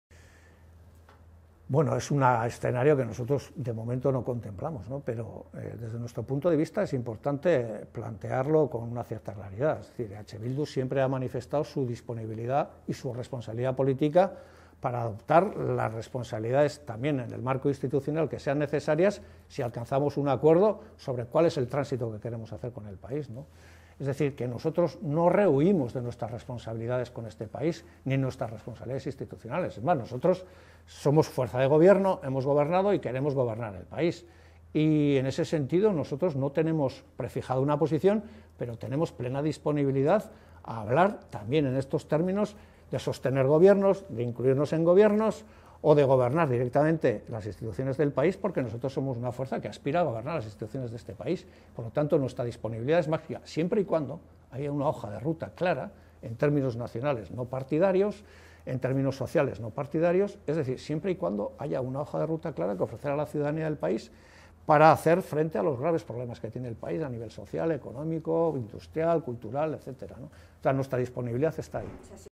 Otegi muestra su "disponibilidad máxima" para respaldar al PNV
El coordinador general de EH Bildu, Arnaldo Otegi, ha mostrado "la disponibilidad máxima" de la coalición soberanista a respaldar al PNV en las instituciones vascas o incluso a entrar en el Ejecutivo vasco si éste se rompe por las divergencias que mantienen los jeltzales con su socio de Gobierno, el PSE-EE, en torno al nuevo estatus para Euskadi.